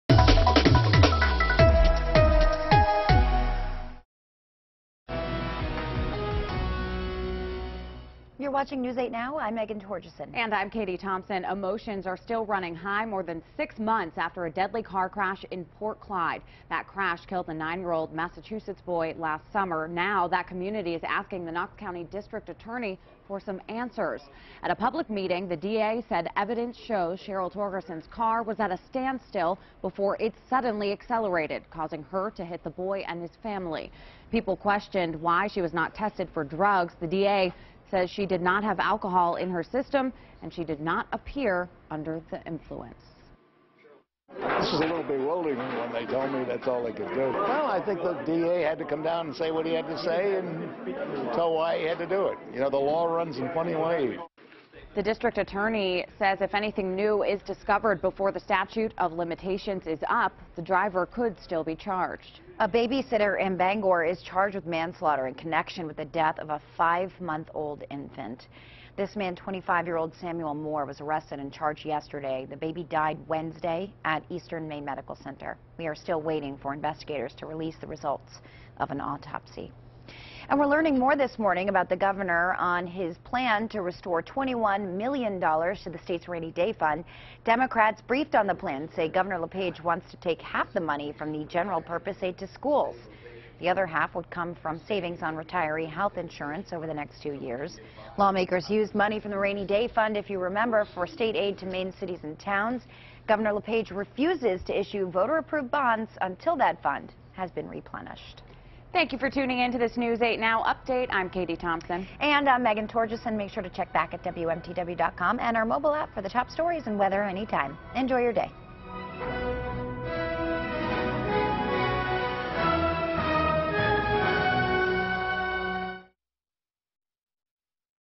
Friday video headlines
WMTW News 8's Katie Thompson and Meghan Torjussen have the latest local headlines for Friday.